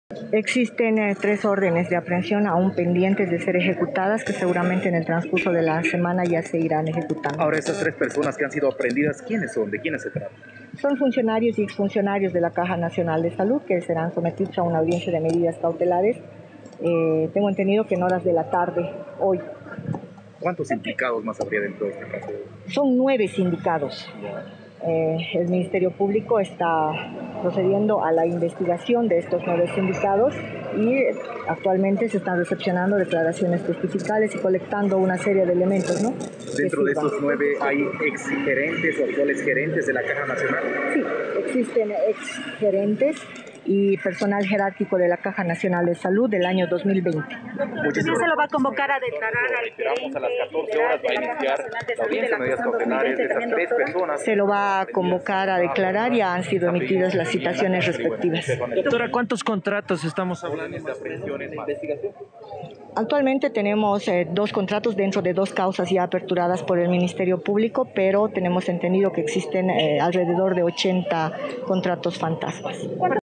Aprehenden a tres personas por el caso "ítems fantasmas" en la CNS
La FELCC de La Paz aprehendió al tercer implicado en el caso de "contratos fantasmas" en la Caja Nacional de Salud (CNS). La fiscal Lupe Zabala aseguró que hay otras tres ordenes de aprehensión pendientes.